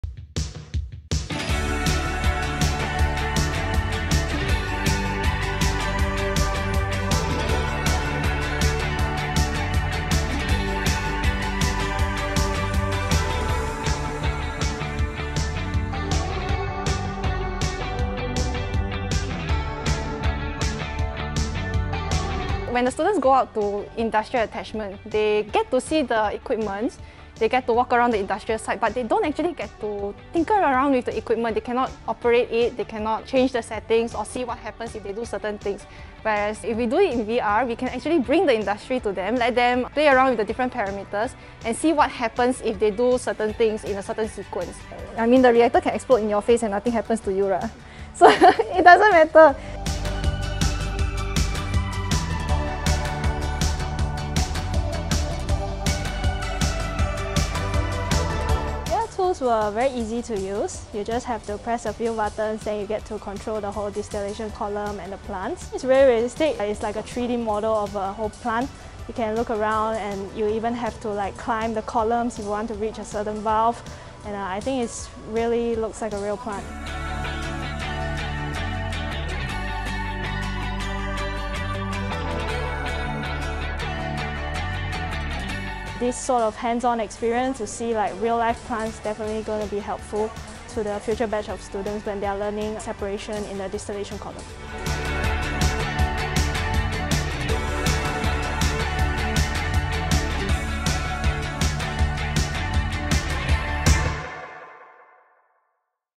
Revolutionizing Engineering Education using Virtual Reality (VR)
NUS Chemical and Biomolecular Engineering Department transforms its classrooms into life-size chemical plants for students to explore and interact with, all by harnessing the power of virtual reality (VR) technology.

Using VR googles, these specially-developed applications help students learn actual handling procedures of chemical plants in the safety of a classroom while also providing a fun and interactive experience.